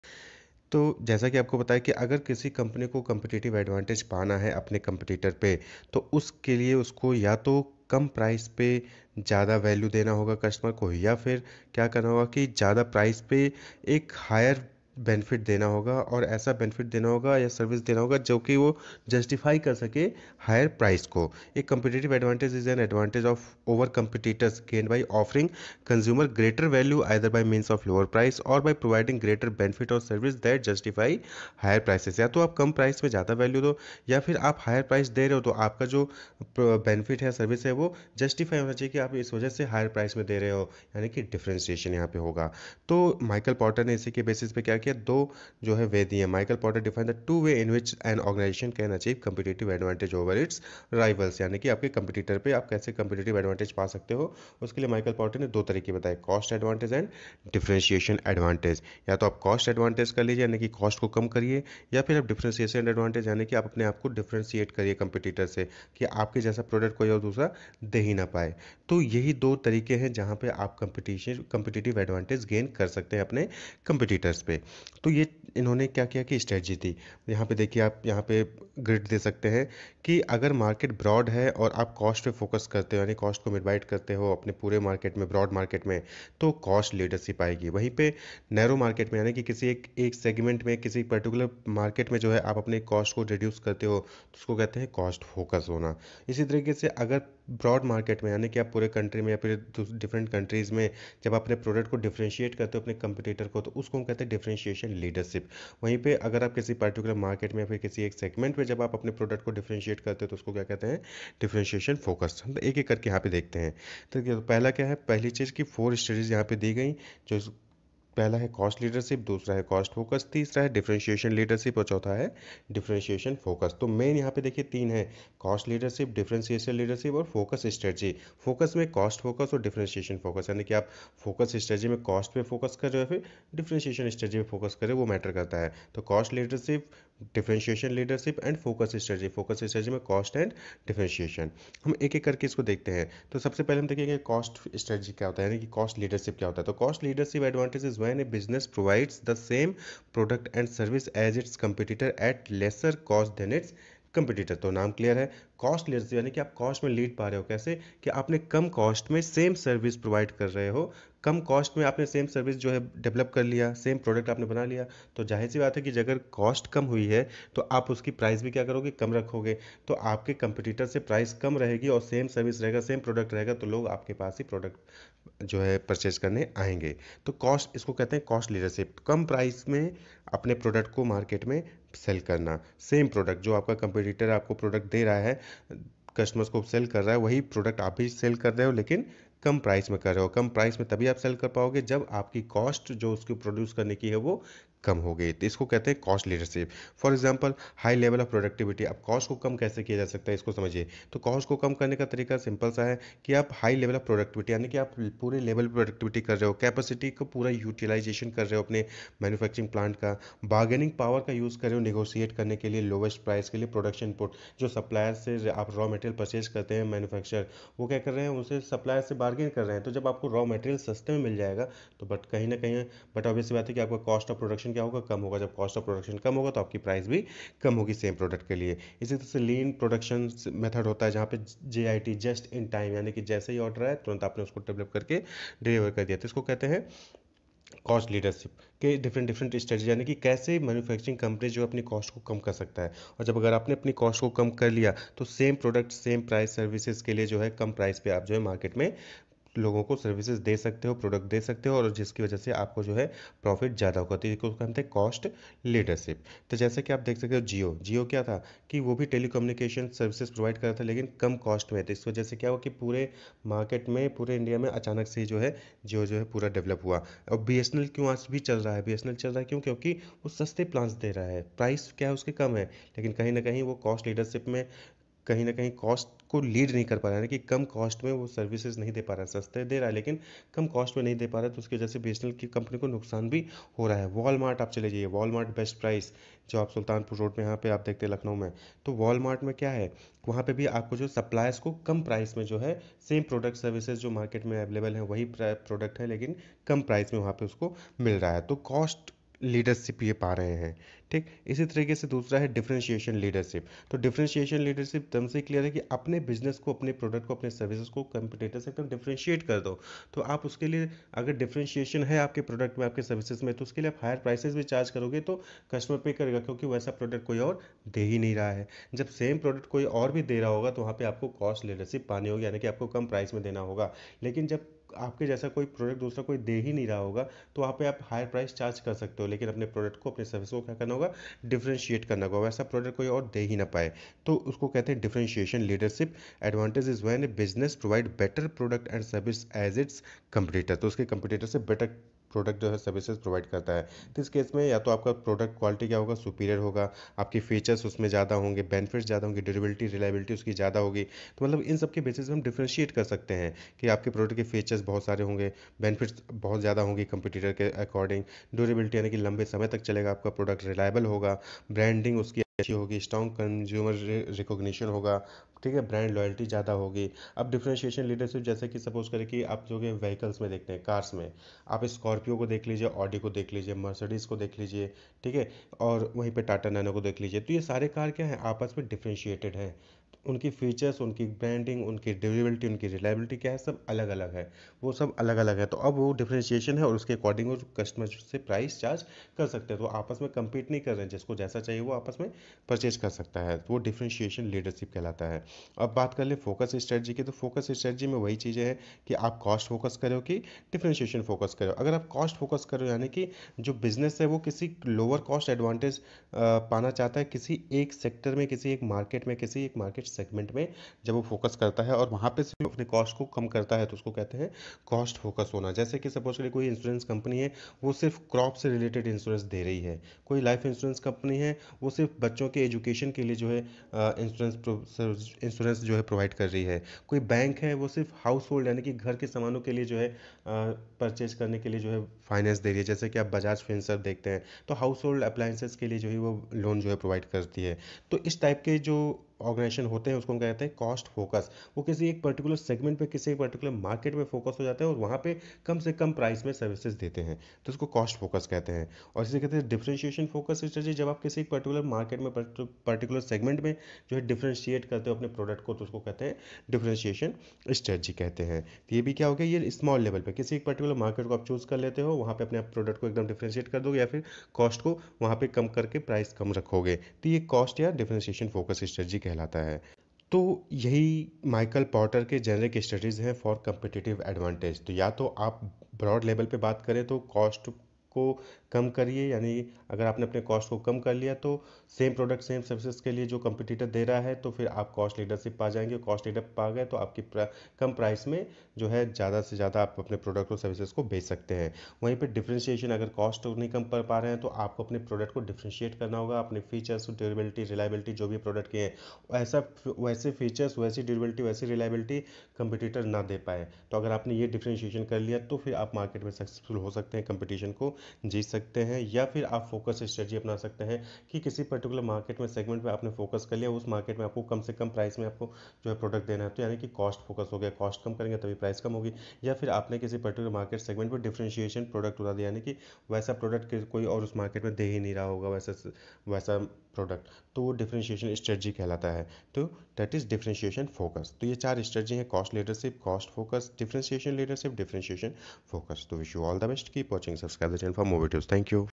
Business Strategies, Porter Generic Strategies, Cost leadership, Competitive Strategies, aktu mba
Playlist of other subjects :
Hello Learner's,
In this video we have quickly revised (Revision फटाफट) discussed following topics of Strategic Management :
Corporate Strategies,
Directional Strategies,
Concentration strategies,
integration strategies,
diversification strategies,